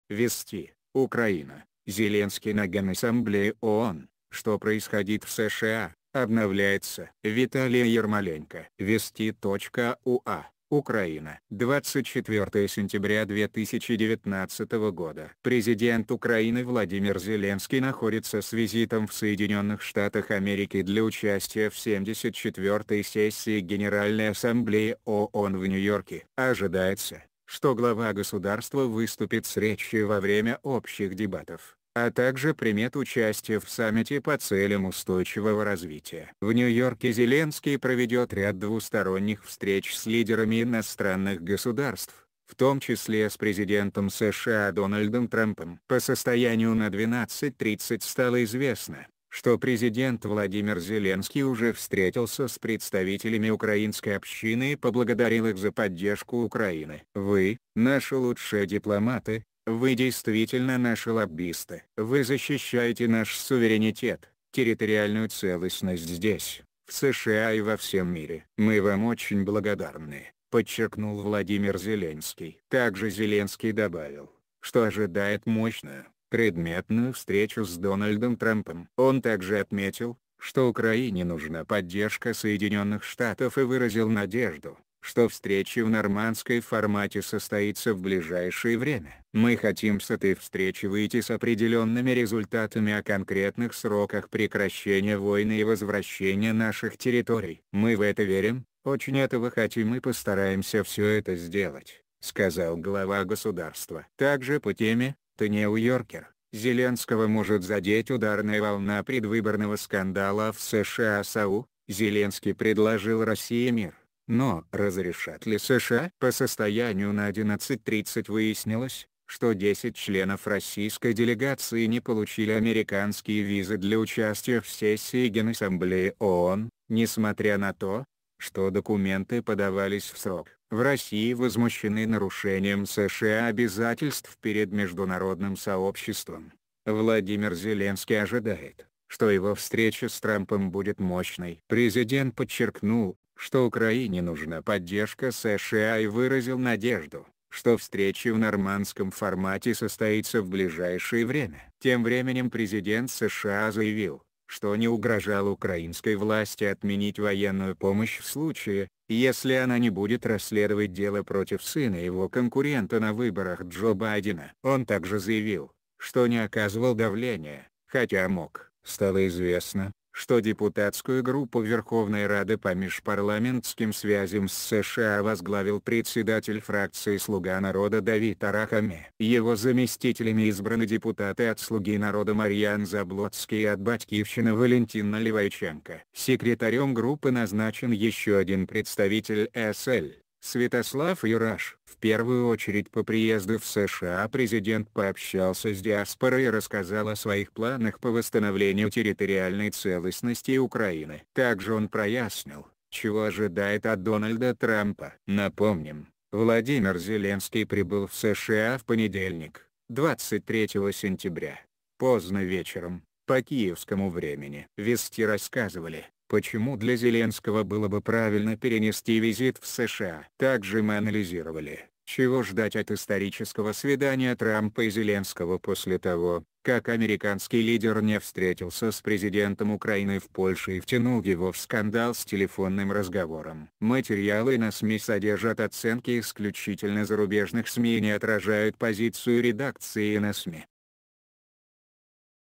Вести (Украина): Зеленский на Генассамблее ООН — что происходит в США (обновляется).
Вести.ua, Украина.
Виталия Ярмоленко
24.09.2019

Президент Украины Владимир Зеленский находится с визитом в Соединенных Штатах Америки для участия в 74-й сессии Генеральной Ассамблеи ООН в Нью-Йорке. Ожидается, что глава государства выступит с речью во время общих дебатов, а также примет участие в саммите по Целям устойчивого развития.  В Нью-Йорке Зеленский проведет ряд двусторонних встреч с лидерами иностранных государств, в том числе с президентом США Дональдом Трампом.  По состоянию на 12:30 стало известно, что президент Владимир Зеленский уже встретился с представителями украинской общины и поблагодарил их за поддержку Украины. «Вы — наши лучшие дипломаты, вы действительно наши лоббисты. Вы защищаете наш суверенитет, территориальную целостность здесь, в США и во всем мире. Мы вам очень благодарны», — подчеркнул Владимир Зеленский. Также Зеленский добавил, что ожидает «мощную, предметную» встречу с Дональдом Трампом. Он также отметил, что Украине нужна поддержка Соединенных Штатов и выразил надежду, что встреча в нормандской формате состоится в ближайшее время. «Мы хотим с этой встречи выйти с определенными результатами о конкретных сроках прекращения войны и возвращения наших территорий. Мы в это верим, очень этого хотим и постараемся все это сделать», — сказал Глава государства.  Также по теме:  The New Yorker: Зеленского может задеть ударная волна предвыборного скандала в США  Sohu: Зеленский предложил России мир, но разрешат ли США?  По состоянию на 11:30 выяснилось, что десять членов российской делегации не получил...